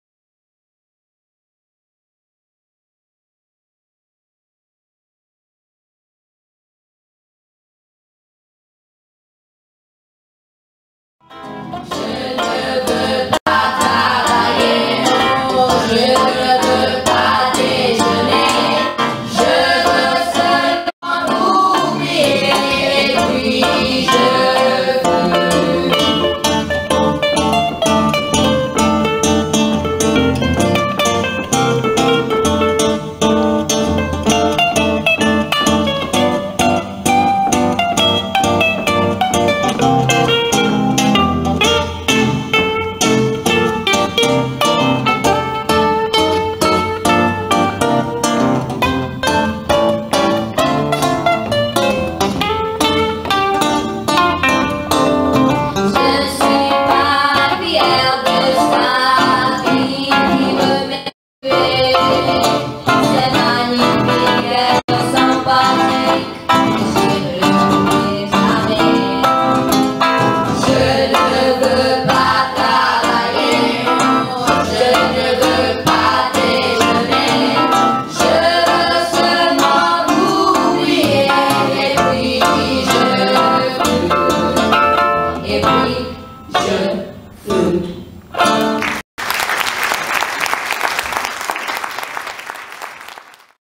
CANTANTI E CHITARRISTI IN TOURNÈE
Degni rappresentanti della Scuola Comunale di Musica di Suzzara all'estero, cantanti e chitarristi, accompagnati da Sara Stacchezzini e Franco Tidona, si sono esibiti al teatro di Brioude (F) in occasione del gemellaggio fra le città.
La nostra musica... espatria.